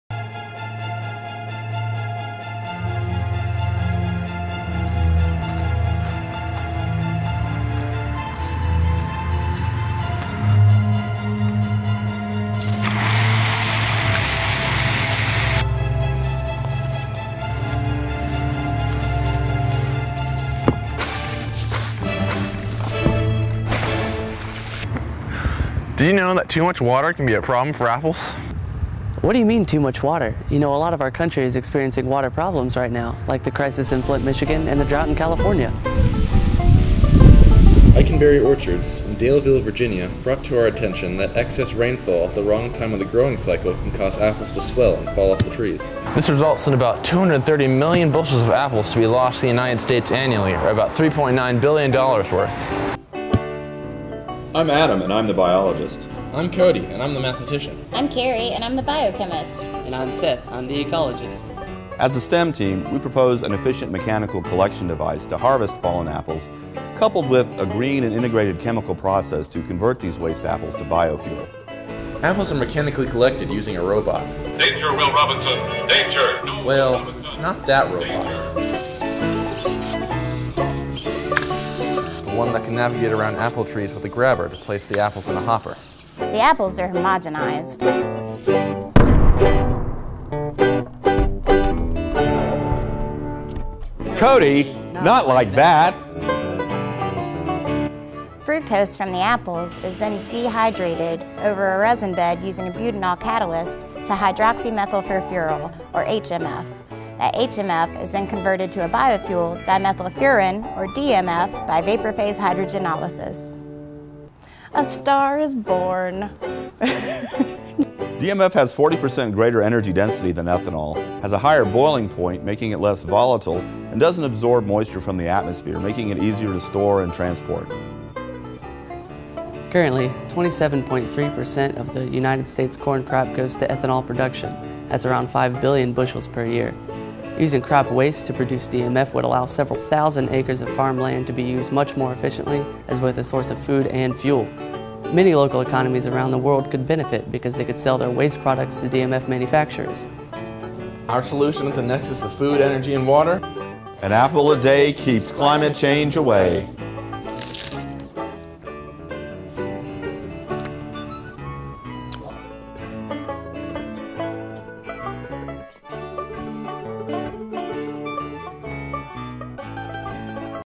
When The Apple Falls
Virginia Western Community College's entry to the 2016 National Science Foundation Community College Innovation Challenge involves an efficient mechanical collection and chemical conversion of waste apples to 2,5-dimethylfuran, a bio fuel with 40% greater energy density than ethanol. Eliminating ethanol crops in the US would free up 13.9 million acres of cropland to be used for food production, driving down food costs globally.